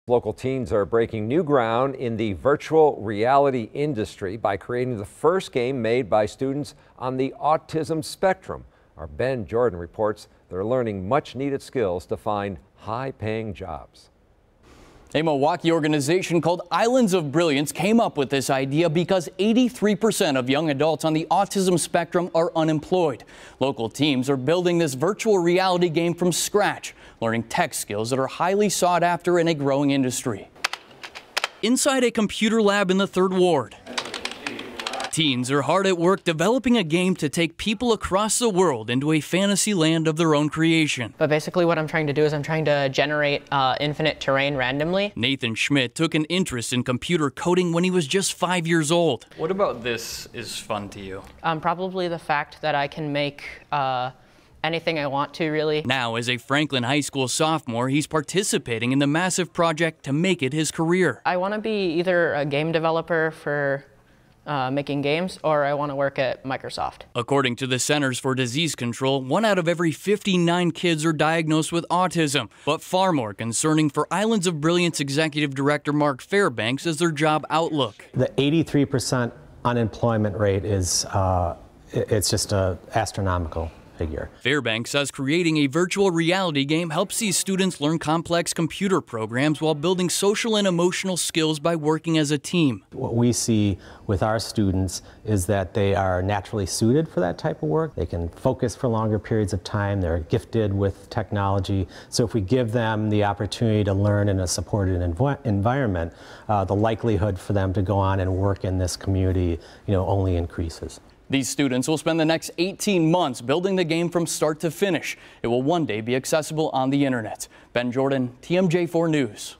Islands of Brilliance: Local teens on autism spectrum break new ground in virtual reality industry
Local teens are building the virtual reality game from scratch, learning tech skills that are highly sought after in a growing industry.